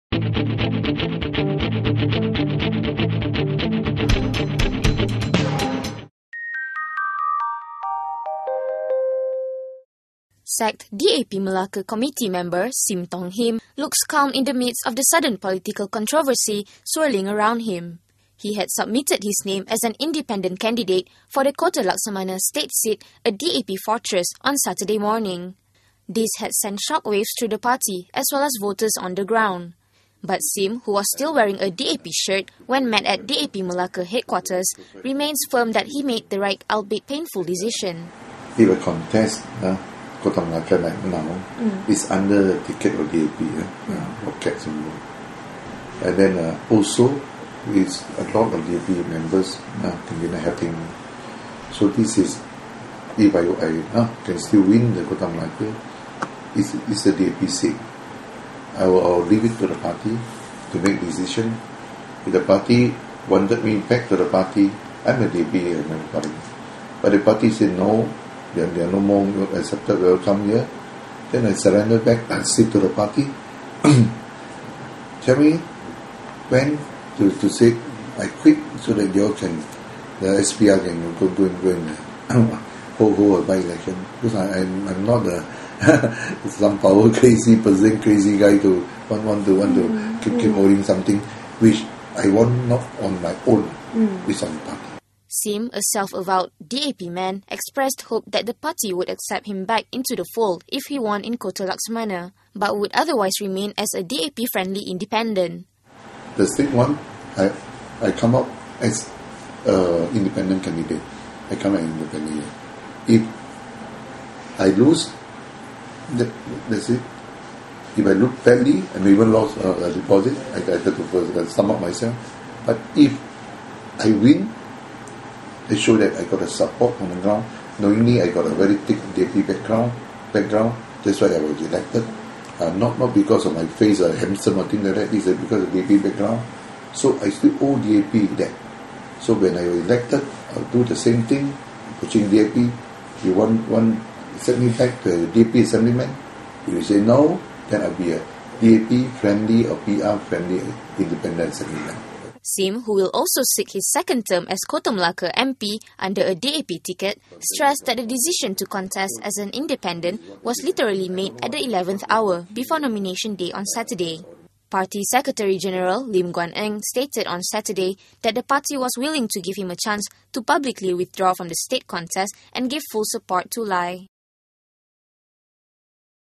GE13: Sim talks about his decision to contest
Sim Tong Him, a self-avowed DAP man, explains why he chose to run not as a DAP candidate for a parliamentary seat but as an Independent for a state seat.